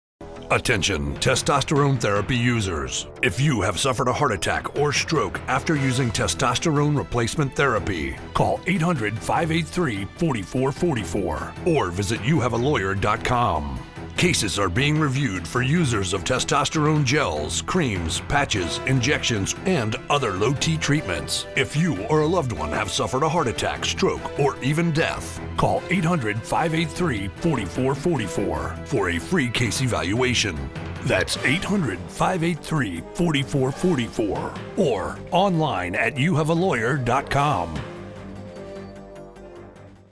Testosterone Therapy Lawyer Advertisement
Saiontz & Kirk, P.A. is reviewing potential testosterone lawsuits for men who suffered a heart attack, stroke, blood clots or death.